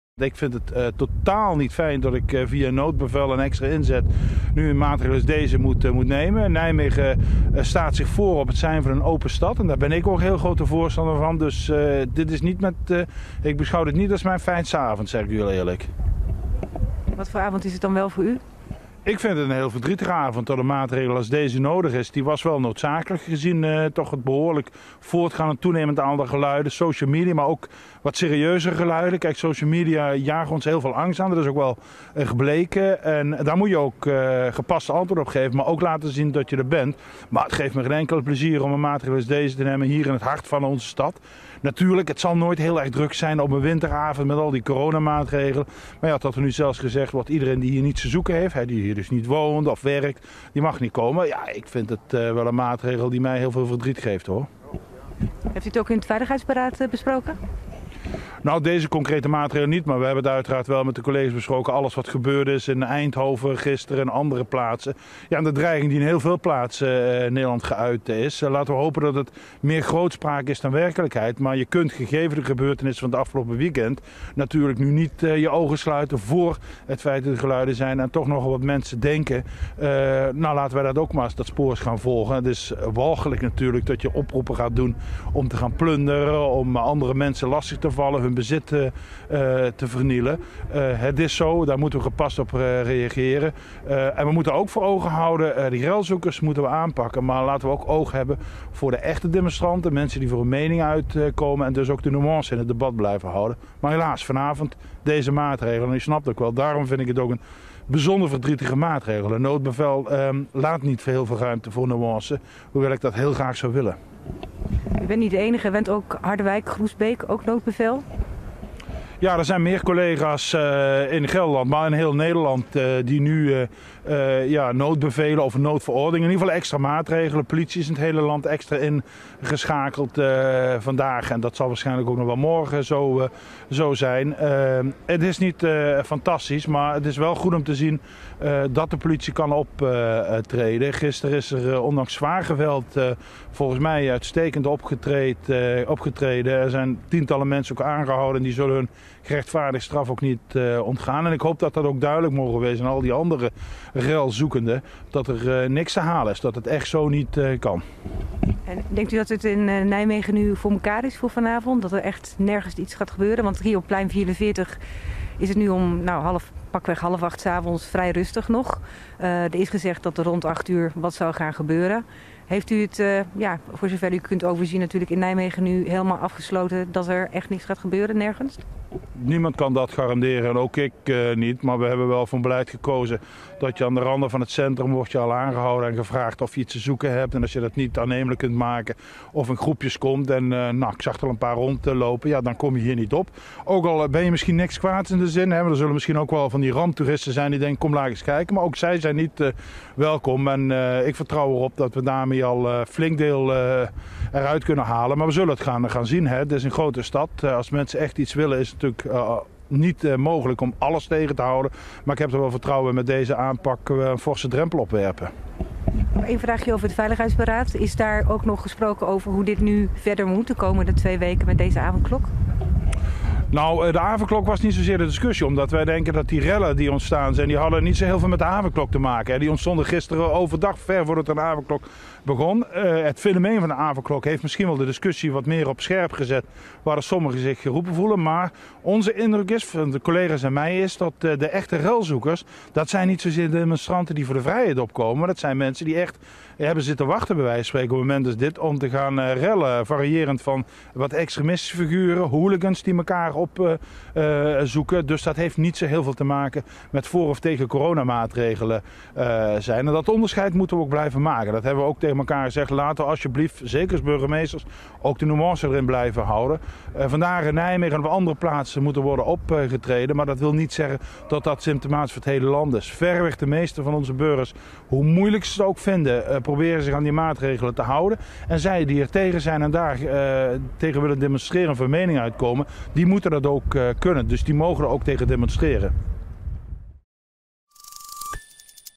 'Dit is een verdrietige avond', zegt burgemeester Bruls over noodbevel
Nijmegen - Burgemeester Hubert Bruls van Nijmegen noemt het een verdrietige avond. Hij doelt op het noodbevel dat hij maandagavond heeft afgekondigd.